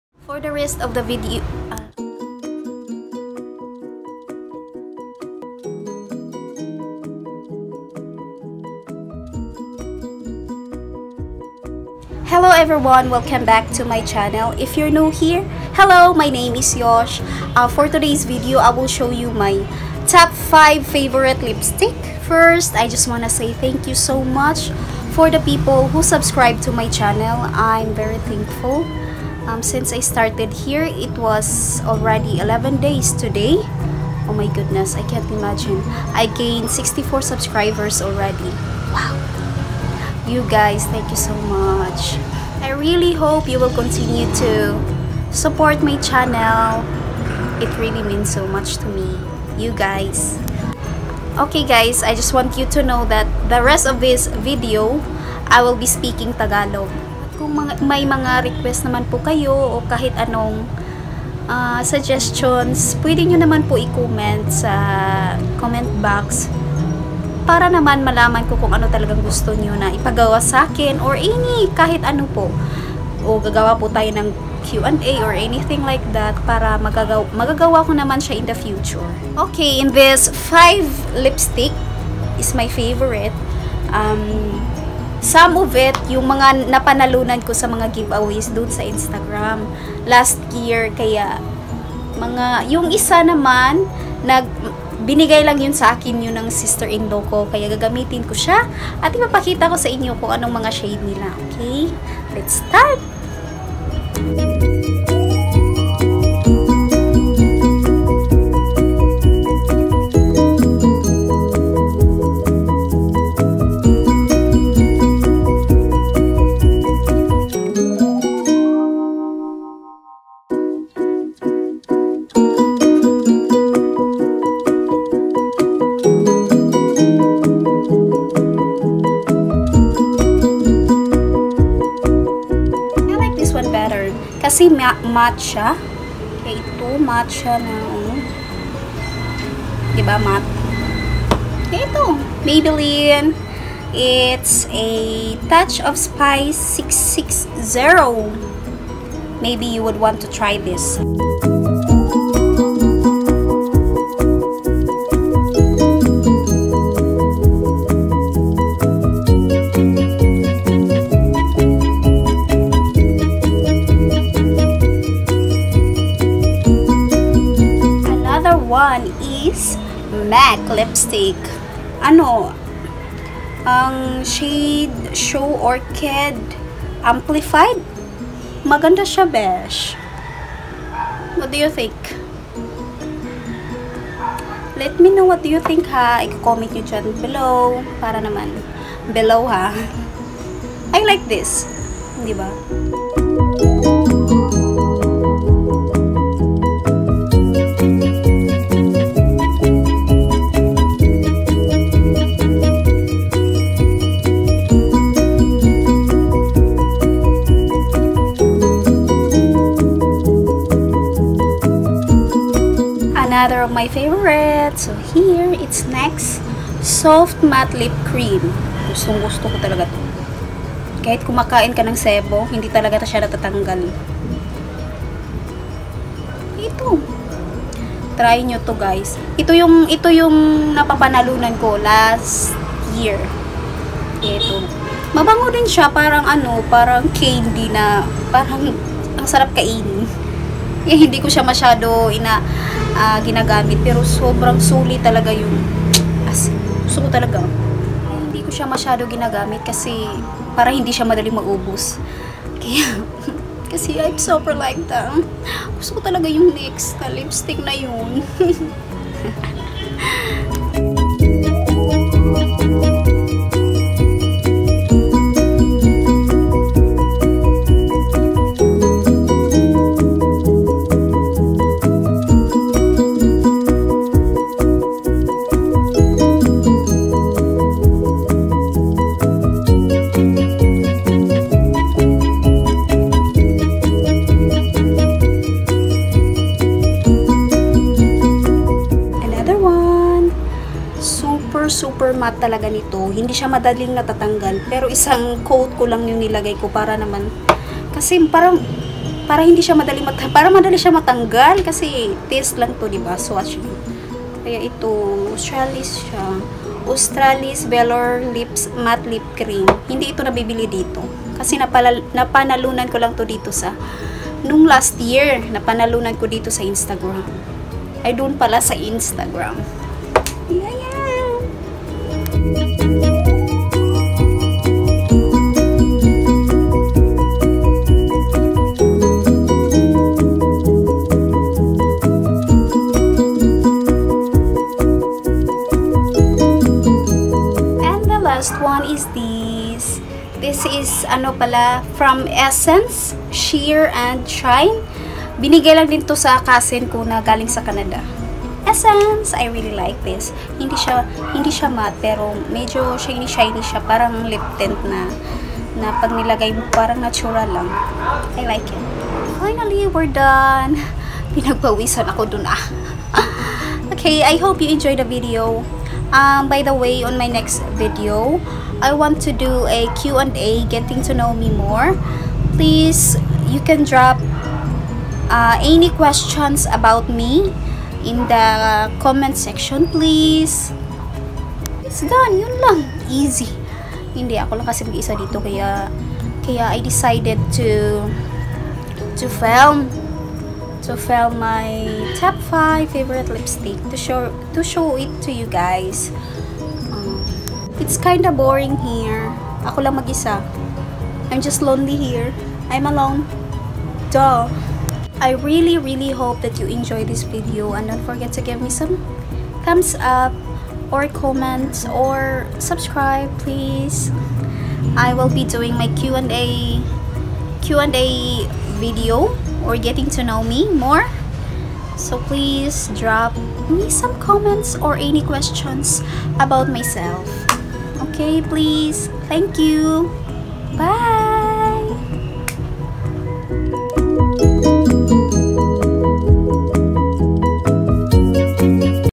My 5 Favorite Lipstick 2018 (TagLish) | Simply Yosh
Hello Gorgeous!
Today is another video I created today.

Btw I want you to drop a comment or questions for my Q&A getting to know me more plssssss! I appreciate it.

Lipstick mentioned:
Matte Lippe- Australis
Soft Matte- Nyx Cosmetics
Sheer&Shine- Essence Cosmetics
Amplified matter- MAC
Touch and spice matte- Maybelline